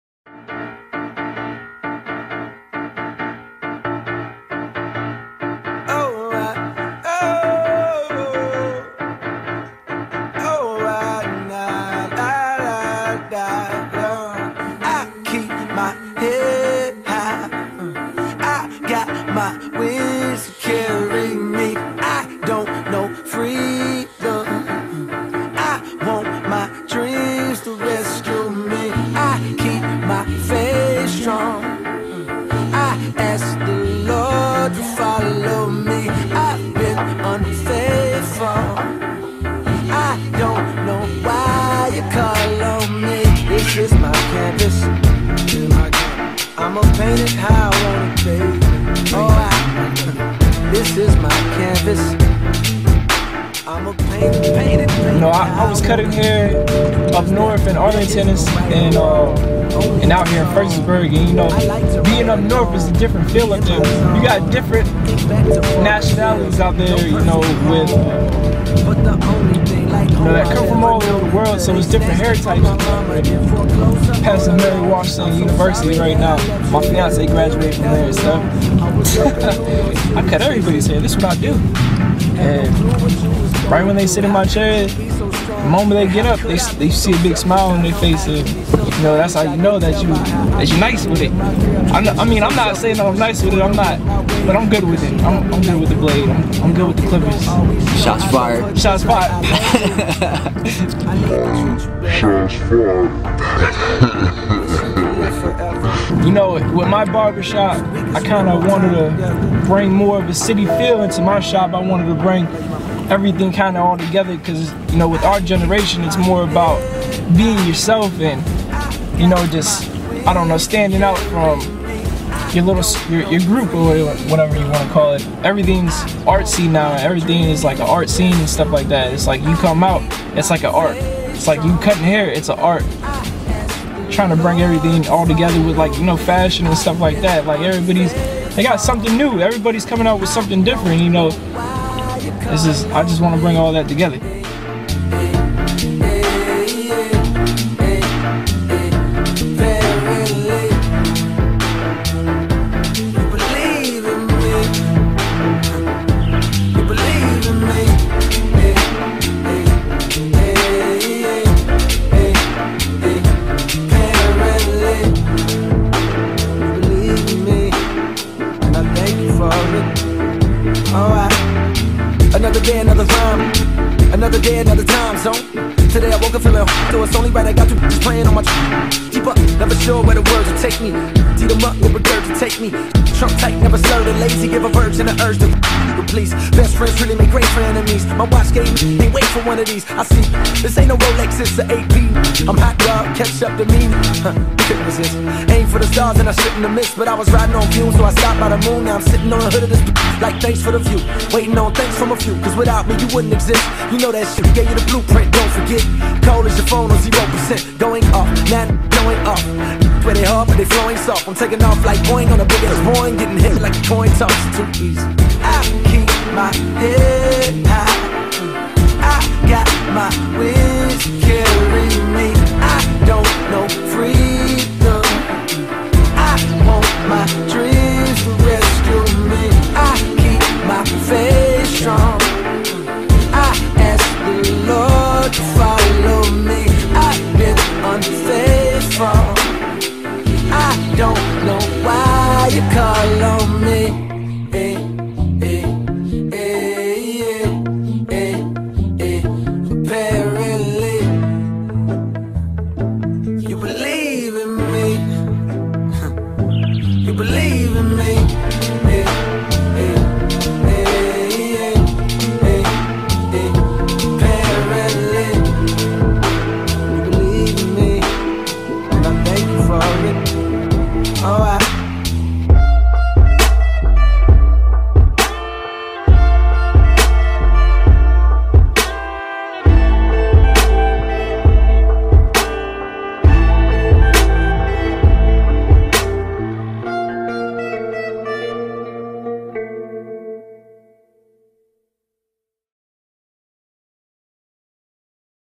THE GENTLEMEN CLUB BARBERSHOP | VLOG EPISODE 1 | FREDERICKSBURG, VA
VLOG for Tone (Owner Of The Gentlemens Club Barbershop) located in Fredericksburg, VA.